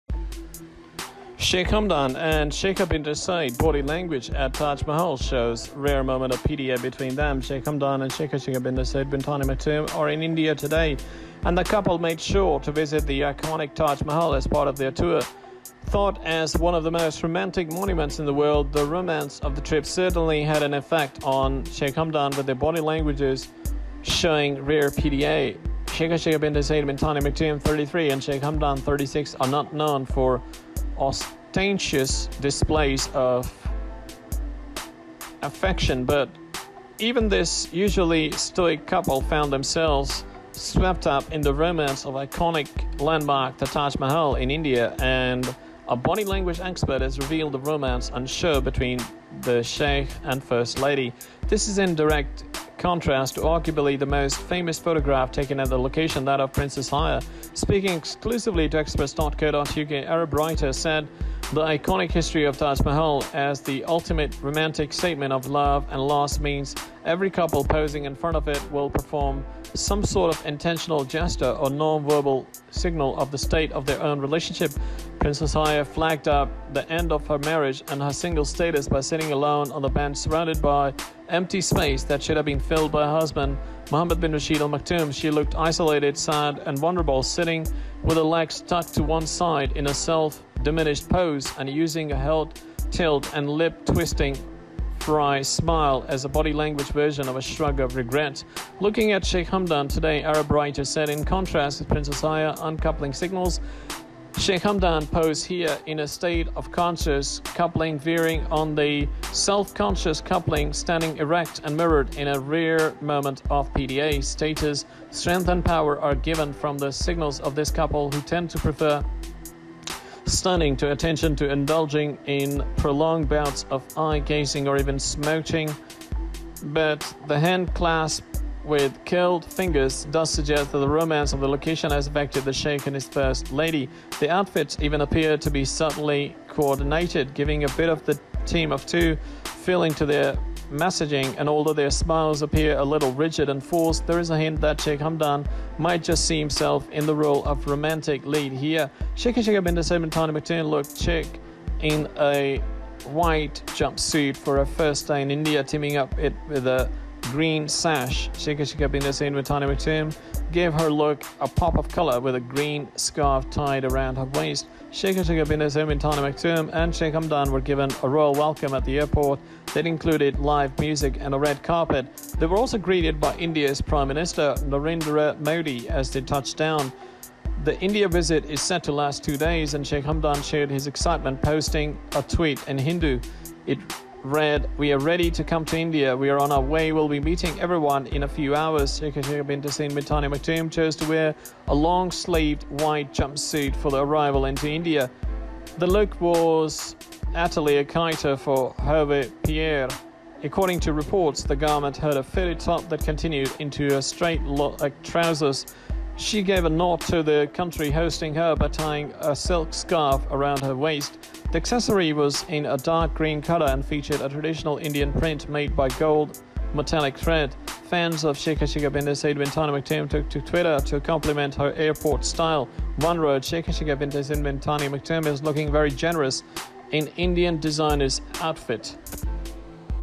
Sheikh Hamdan & Sheikha bint Saeed body language at Taj Mahal shows rare moment of PDA between them
sheikh hamdan and Sheikha Sheikha bint Saeed bin Thani Al Maktoum are in India today, and the couple made sure to visit the iconic Taj Mahal as part of their tour.